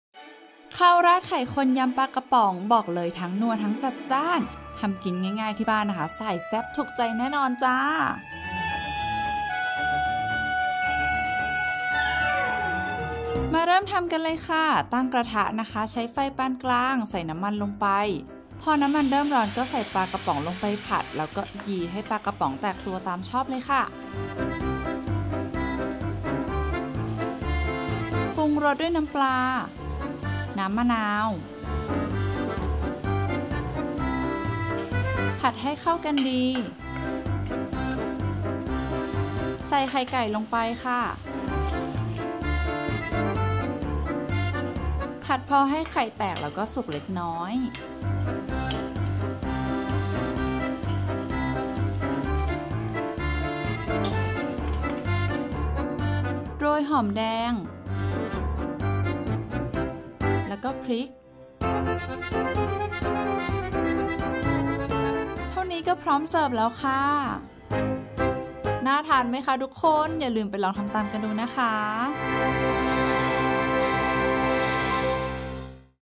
ข้าวราดไข่คนยำปลากระป๋อง
ข้าวราดไข่คนยำปลากระป๋อง เมนูเอาใจคนรักความแซ่บ!
บอกเลยทั้งนัว ทั้งจัดจ้าน ถูกใจแน่นอนจ้า 🥰🥰

✨ทุกคนน ไหนใครชอบทานข้าวไข่คนบ้างคะ แป้งชอบมาก แต่วันนี้แอบมาเพิ่มความจัดจ้านให้ไข่คนด้วยยำปลากระป๋องแซ่บๆ เข้ากันสุดๆไปเลยค่า